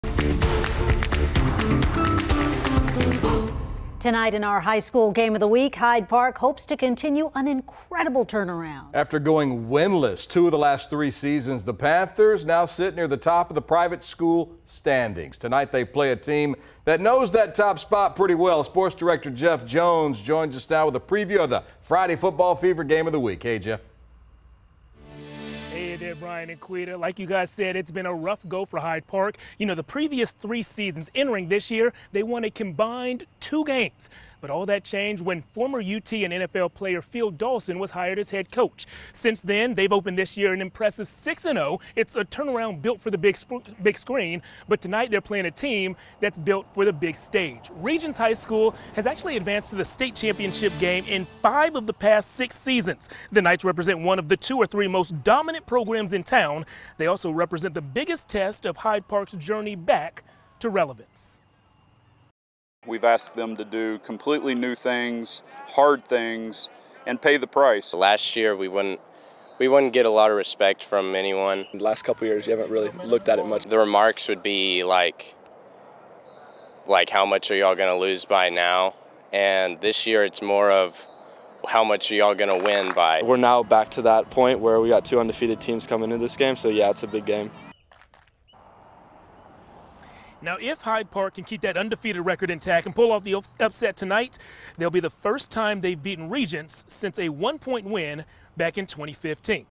Game of the Week: Hyde Park vs. Regents | KVUE
Hyde Park hopes to continue an incredible turnaround. After going winless in two of the past three seasons, the Panthers now sit near the top of the private school standings.